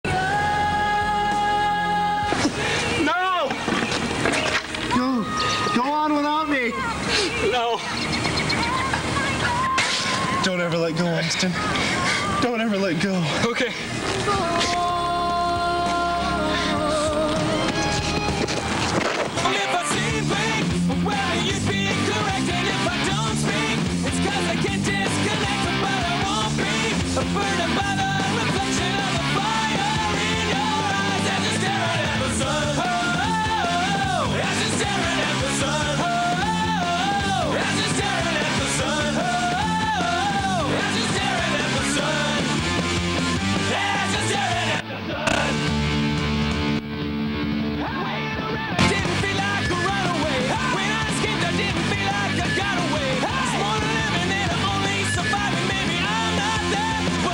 SN Road Trip Part 4
Directed By: Brian ::: Year: 2000
Filmed By: Cast
Starring: Lightning, Austin, Brian, & Rob
Comment: We made a news broadcast while driving, & in between the news we had a side story.  This clip is the end of the episode.  It was originaly made a for a Channel One competition, but back in the day of VHS, the generation loss was horrible & probably killed our chance on that aspect alone.